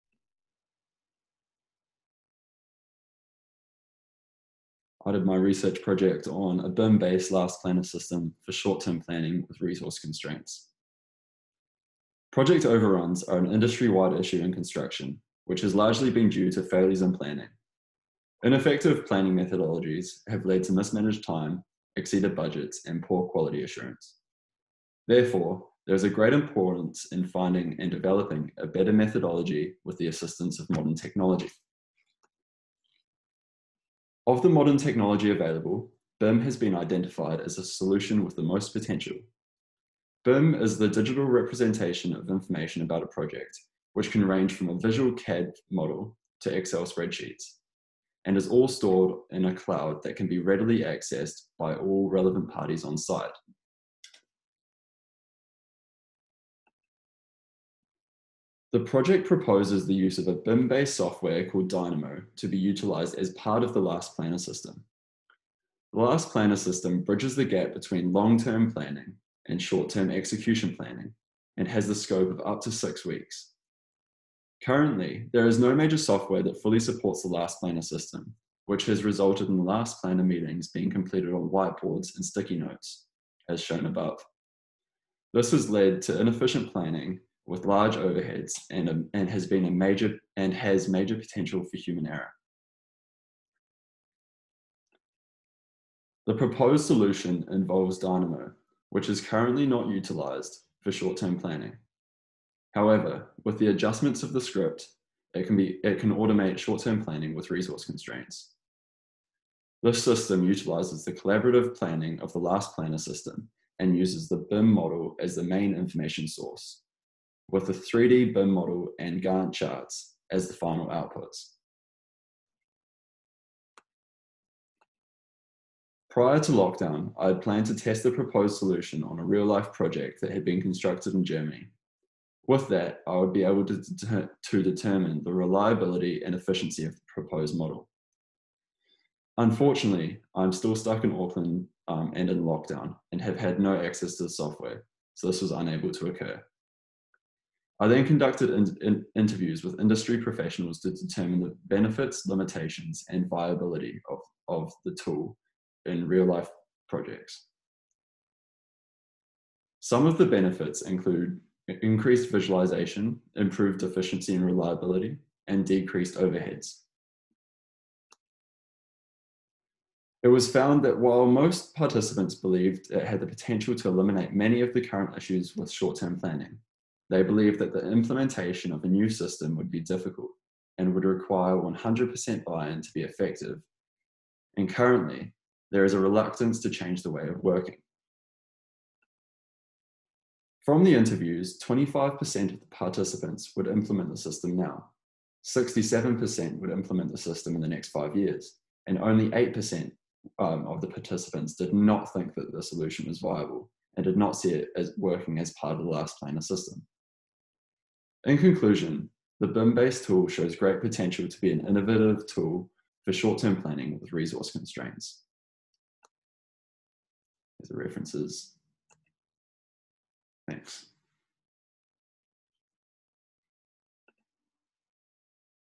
BIM-Based Last Planner System for Short-Term Planning with Resource Constraints
R&D project by: Jamie McInnes
Supervisors: Ray Hudd

BE(Hons) research & development project.
Waikato Engineering Design Show, 2021, School of Engineering & School of Computing and Mathematical Sciences, The University of Waikato.